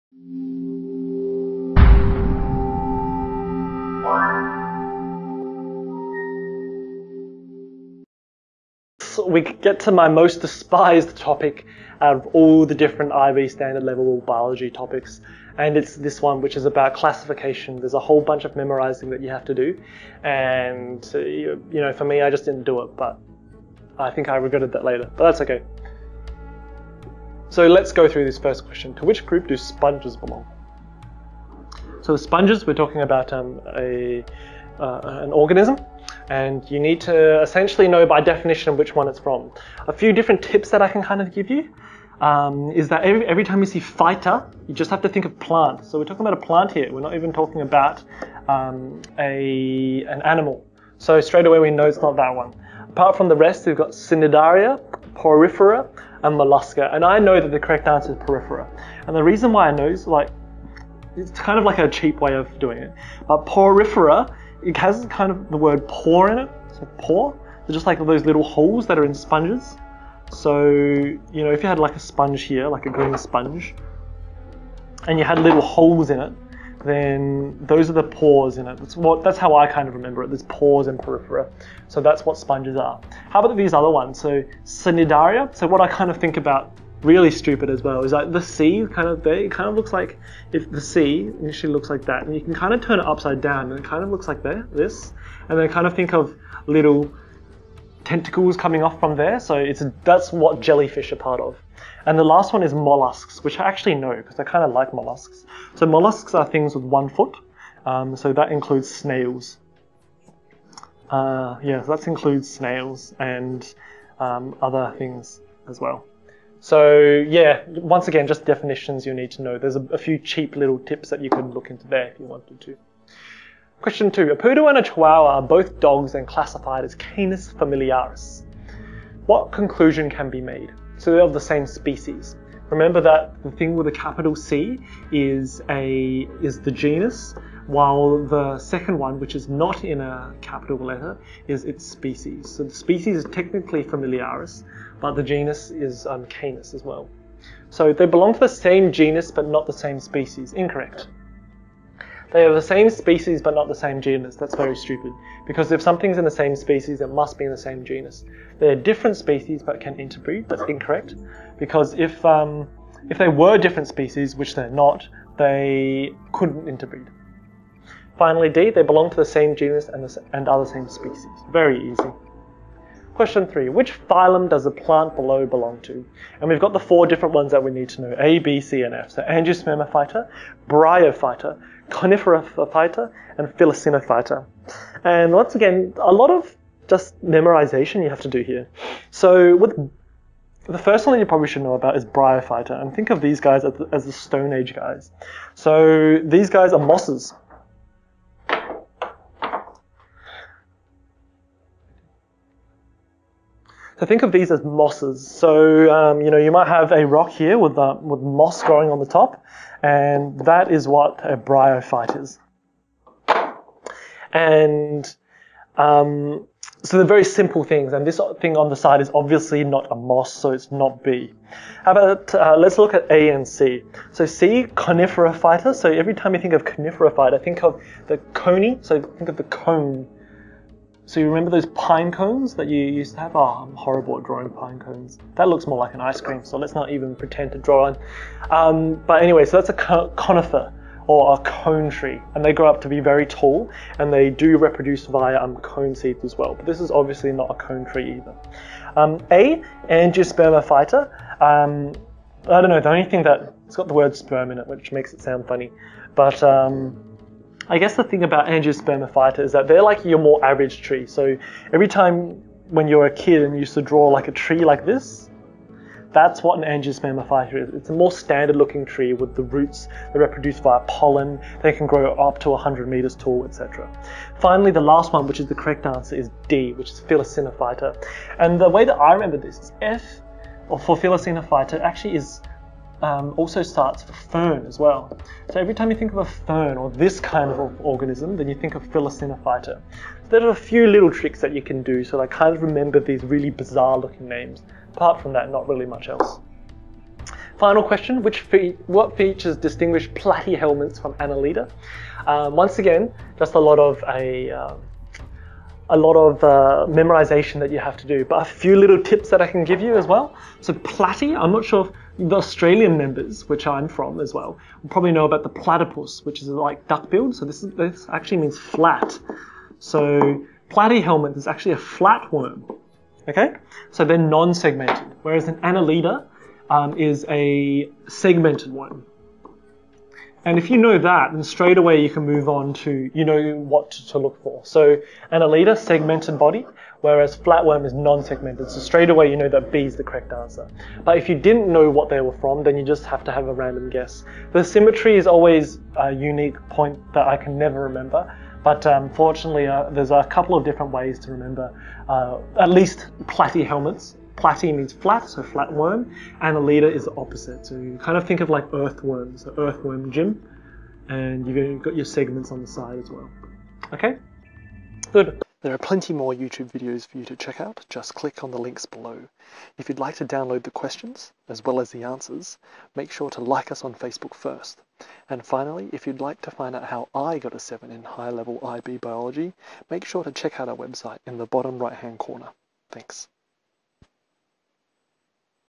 Classification - IB SL Biology Past Exam Paper 1 Questions
Standard Level IB Biology Past Paper 1 Exam questions. Please comment on what else you'd like me to do, and I'll try and do it :)

Find out how I get a 7 in HL biology using my 2 step plan - go to my